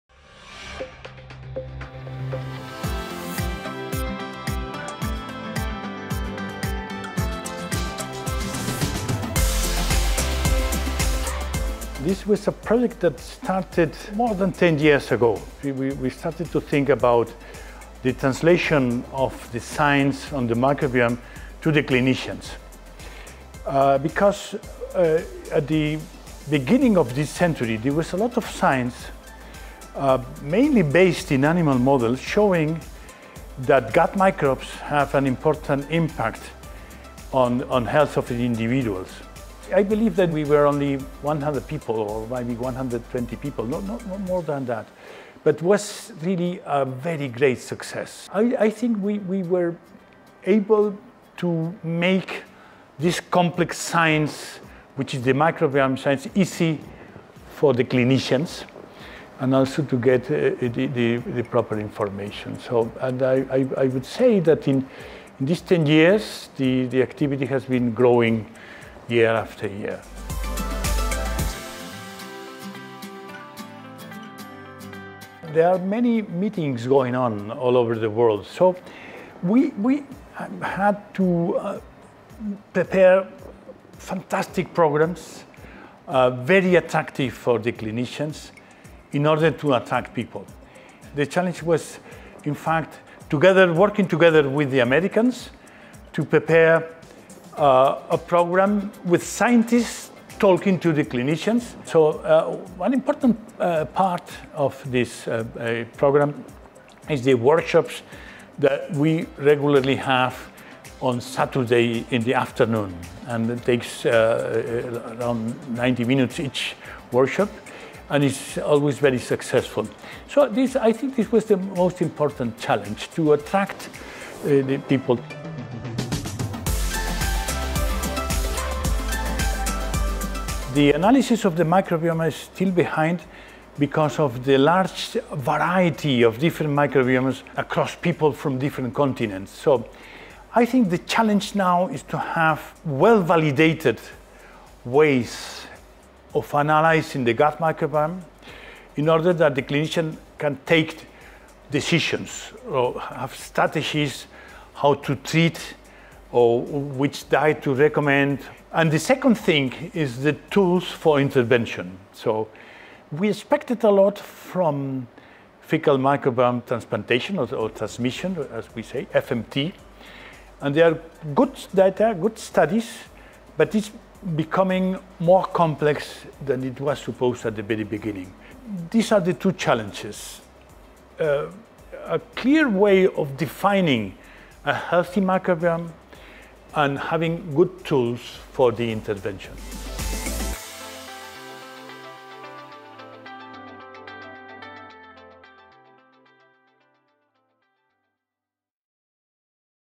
GMFH10years: 10 años de la Cumbre Mundial sobre Microbiota Intestinal para la Salud
“Pudimos hacer que esta ciencia compleja, que es la ciencia del microbioma, sea fácil para los médicos”. Francisco Guarner nos cuenta sobre los primeros días de la Cumbre Mundial GMFH y los desafíos para el futuro de la ciencia de la microbiota intestinal.